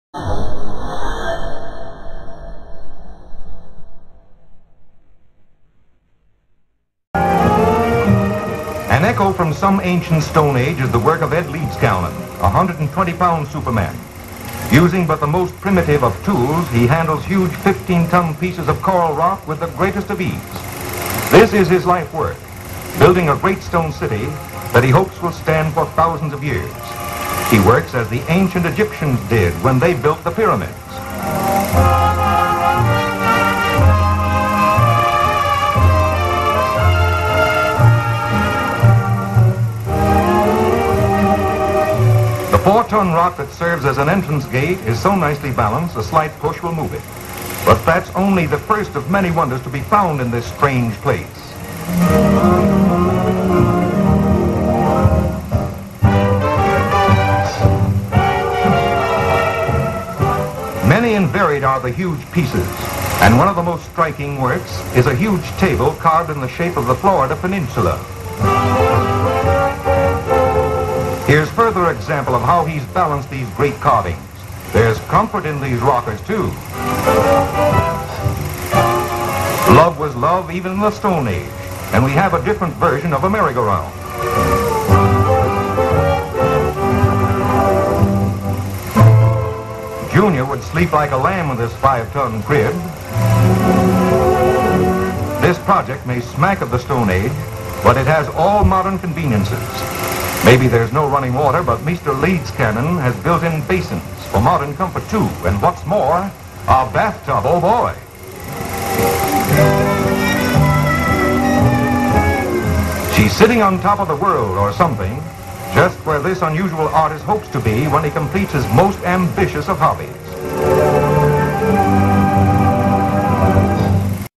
VERY rare footage of Coral Castle and Edward Leedskalnin
Coral Castle is a stone structure created by the Latvian American eccentric Edward Leedskalnin (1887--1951) north of the city of Homestead, Florida in Miami-Dade County at the intersection of South Dixie Highway  and SW 157th Avenue. The structure comprises numerous megalithic stones (mostly limestone formed from coral), each weighing several tons. It currently serves as a privately operated tourist attraction. Coral Castle is noted for legends surrounding its creation that claim it was built single-handedly by Leedskalnin using reverse magnetism and/or supernatural abilities to move numerous stones weighing many tons and carving them.

According to the Coral Castle's own promotional material, Edward Leedskalnin was jilted by his 16-year-old fiancée Agnes Scuffs in Latvia, just one day before the wedding. Leaving for America, he came down with allegedly terminal tuberculosis, but spontaneously healed, stating that magnets had some effect on his disease.